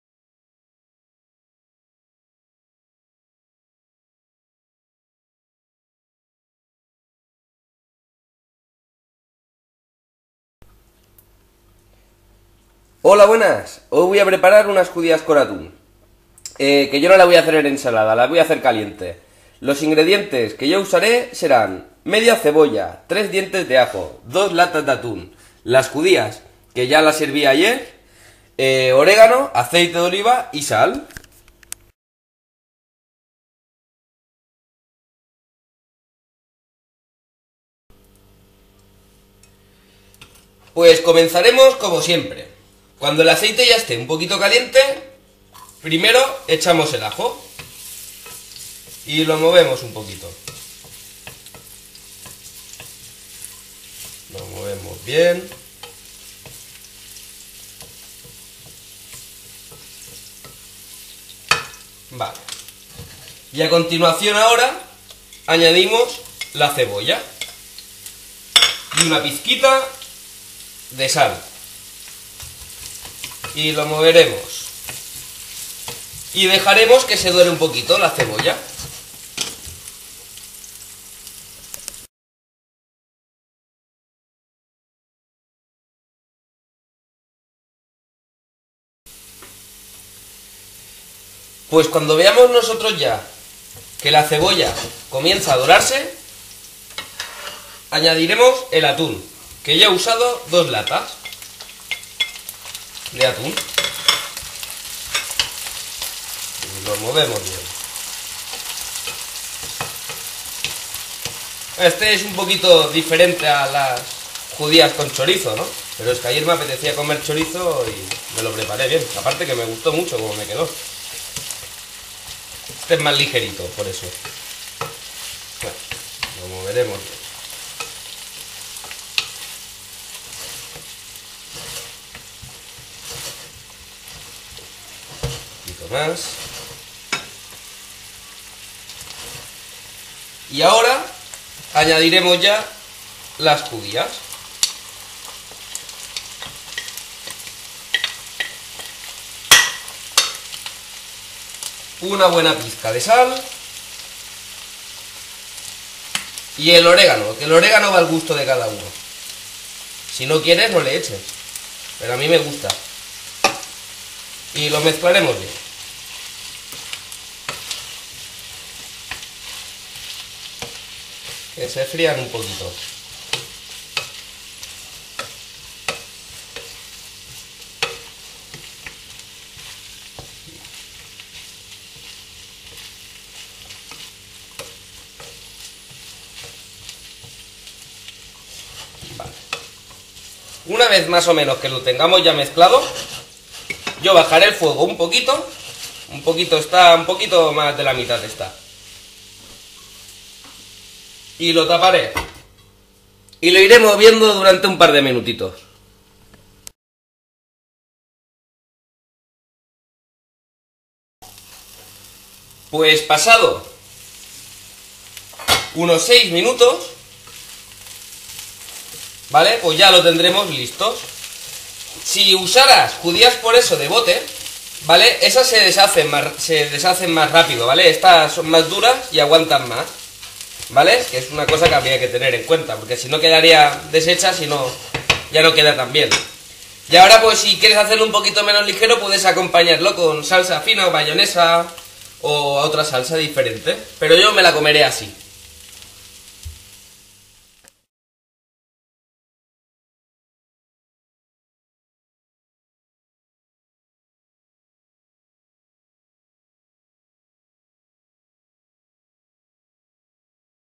Judías con atún
Un plato sencillo y con ingredientes básicos, fácil y rápido de preparar ;)
Ingredientes:
Judías 3 Dientes de ajo Media cebolla 2 Latas atún Orégano Aceite de oliva Sal.